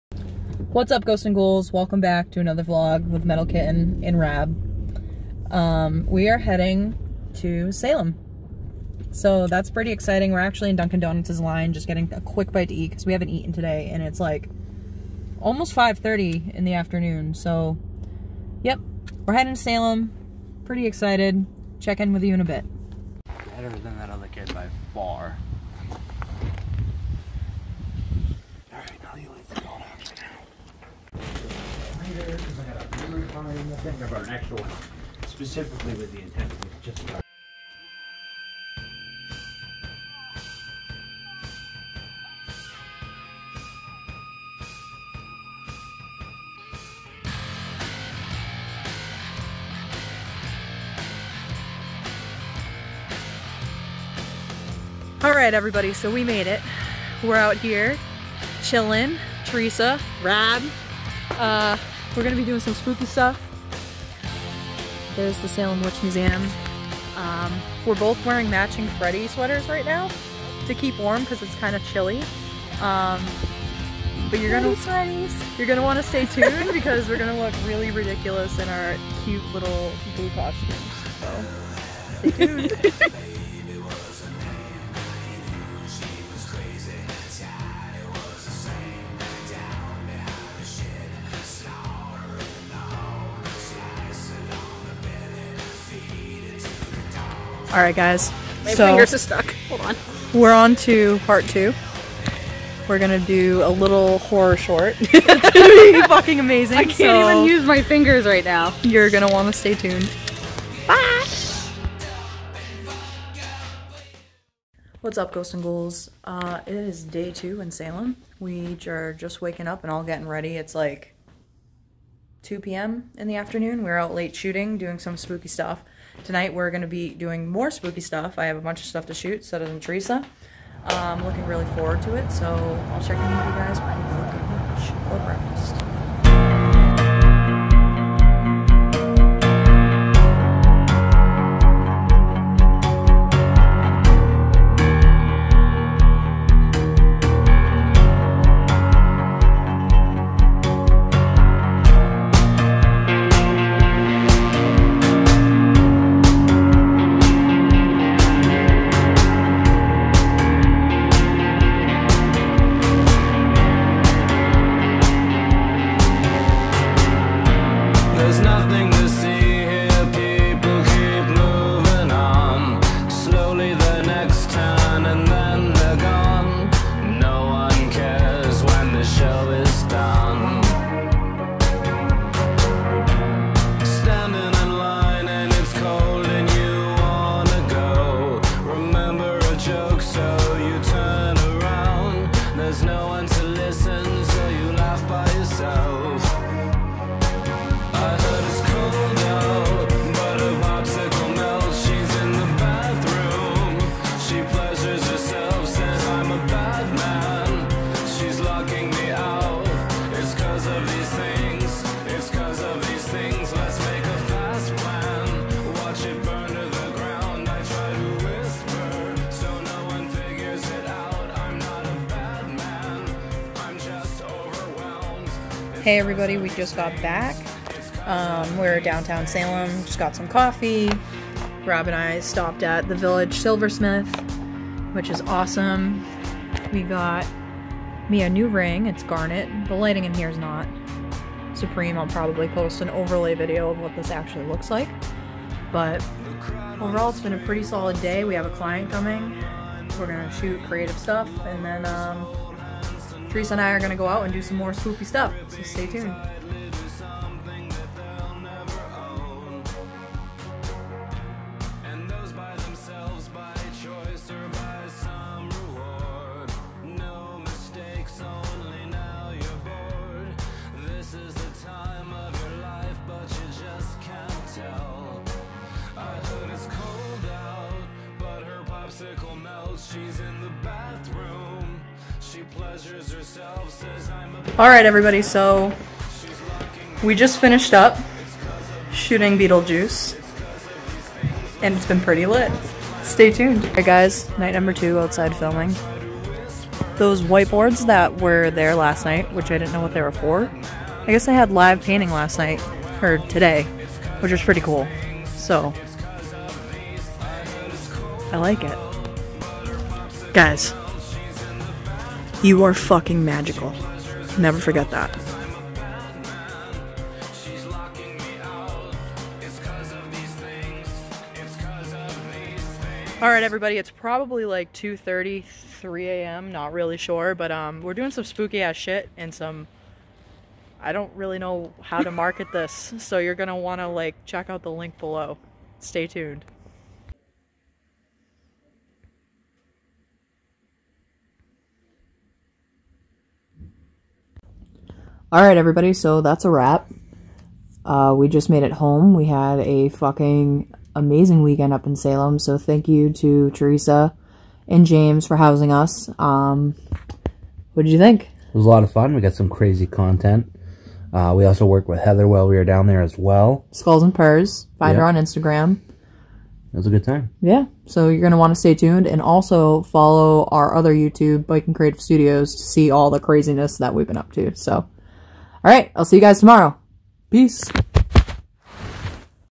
Why I got arrested in Salem
Join me on another adventure up to our sister studio up in Salem, Ma! I always find myself in interesting predicaments...

Support what we do in the link below or to see uncensored content we create!